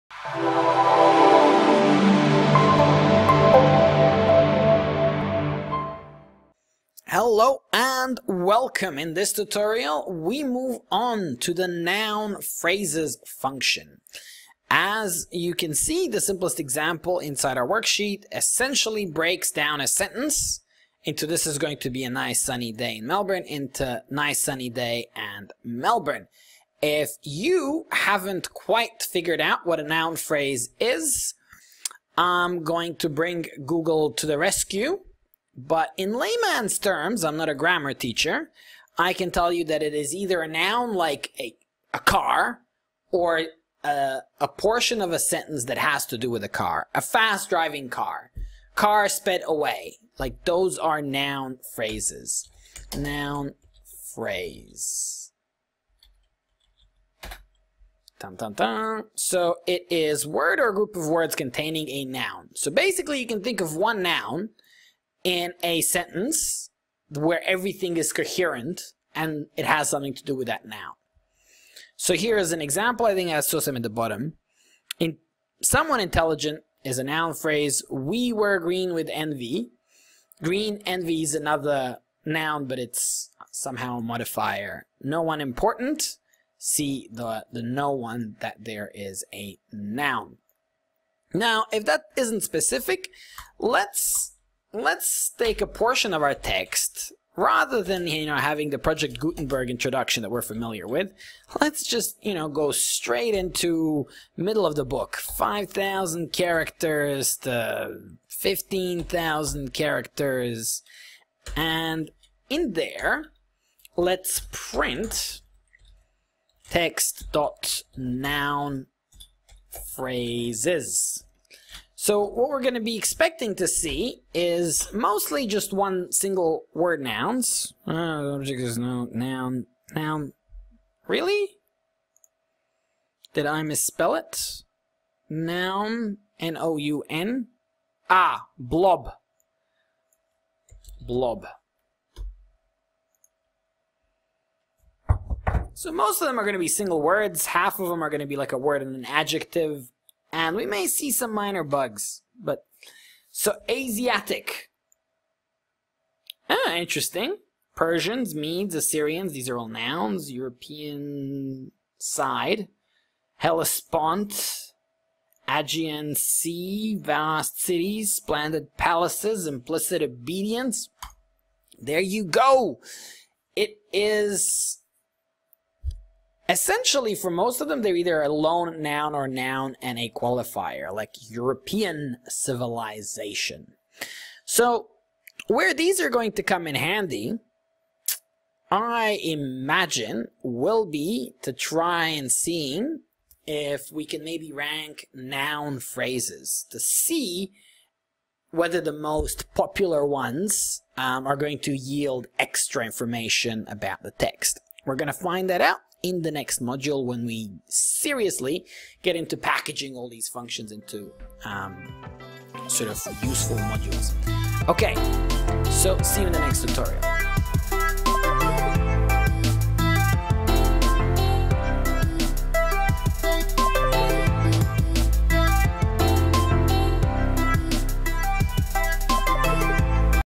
Generating a list of noun_phrases using TextBllob
This tutorial will show you how to split up a large amount of text into noun phrases.

A noun phrase is either a pronoun or any group of words that can be replaced by a pronoun. For example, 'they', 'cars', and 'the cars' are noun phrases, but 'car' is just a noun, as you can see in these sentences (in which the noun phrases are all in bold) Q: Do you like cars?

WEBSITES
Headstartacademy.com.au
Techxellent.com.au
Robotix.com.au